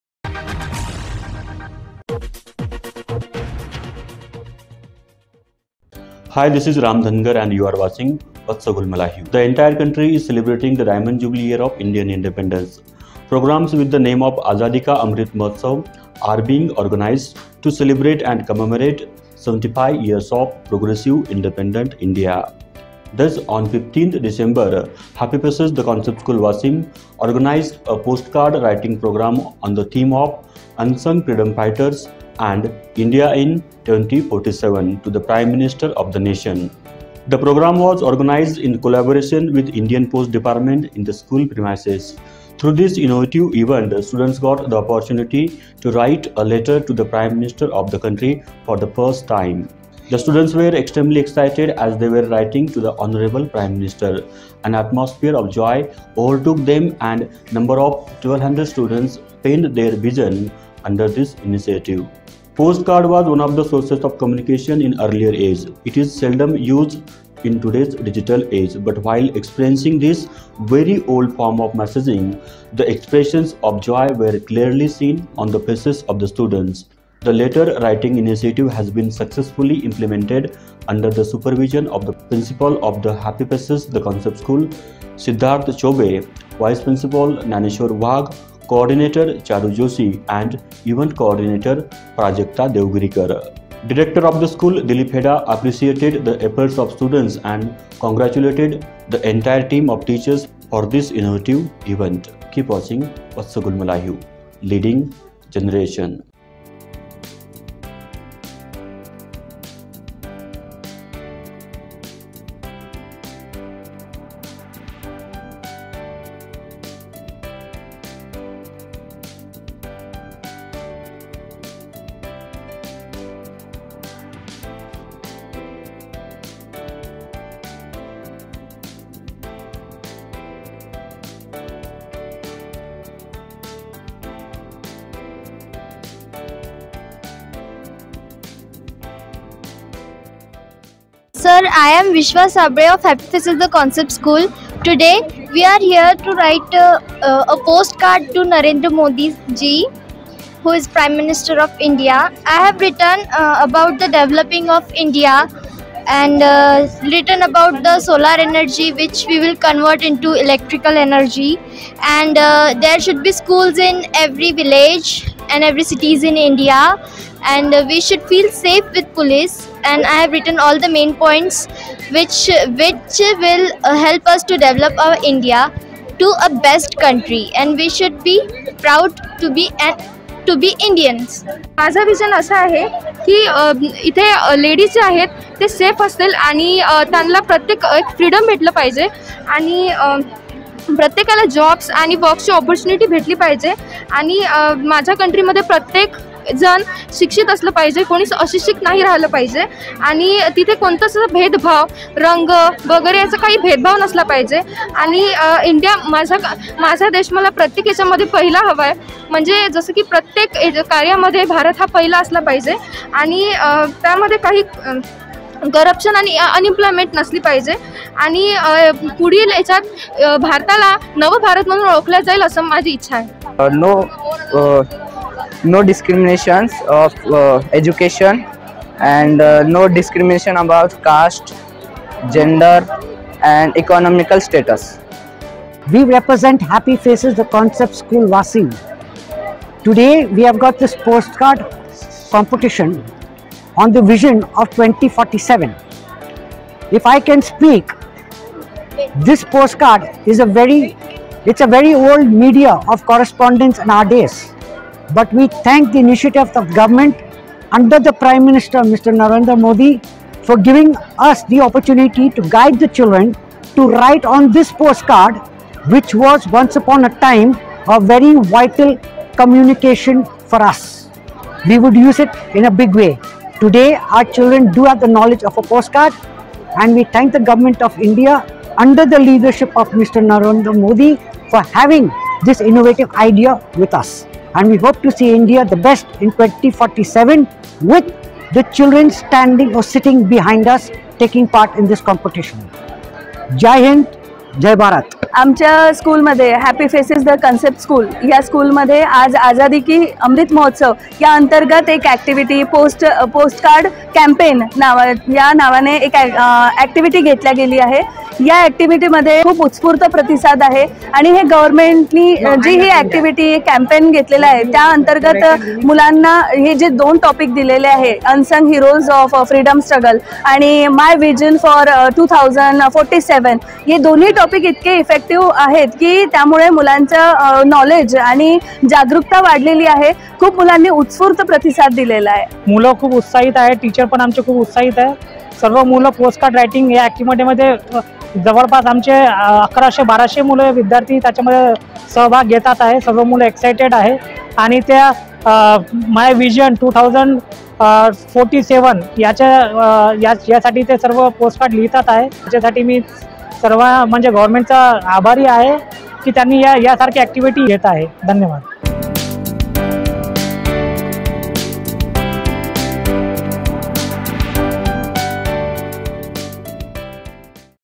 What do the students expect from Prime Minister Modi? Students of Happy Faces sent 1200 Letters.
Leading Generation.
For News,Interview,Success Story and advertisement